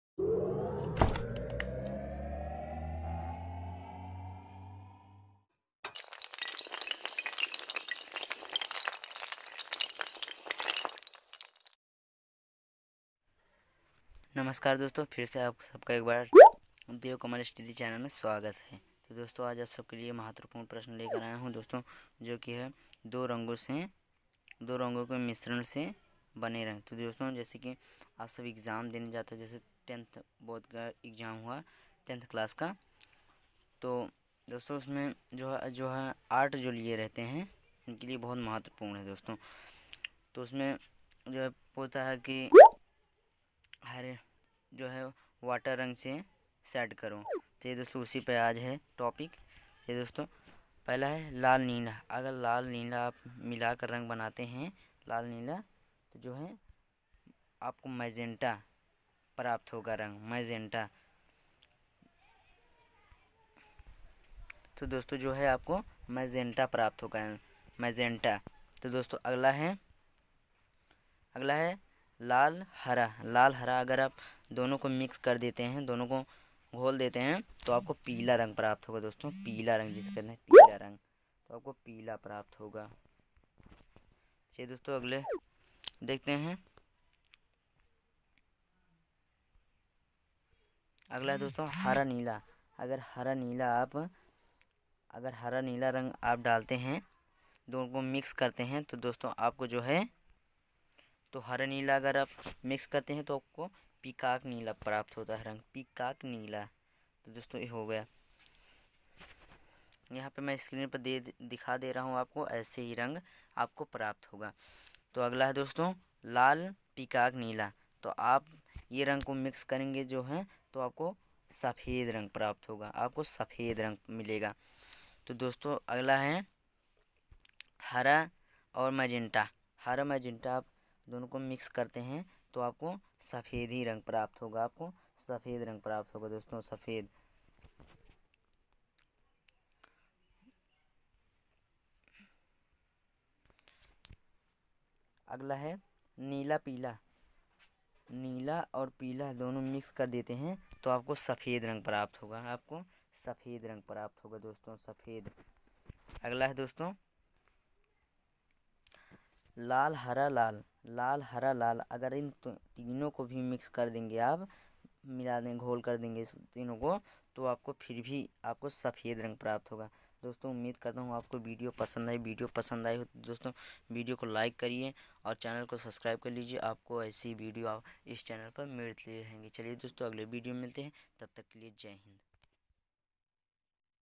Colour Mixing Short Tricks || दो रंग को मिलाने के बाद कौन सा रंग बनता है
Colour Mixing Short Trick .दो रंगों के मिश्रण से बने रंग, दो Colour को मिलाकर  कौन सा Colour बनता है

Very knowledgeable video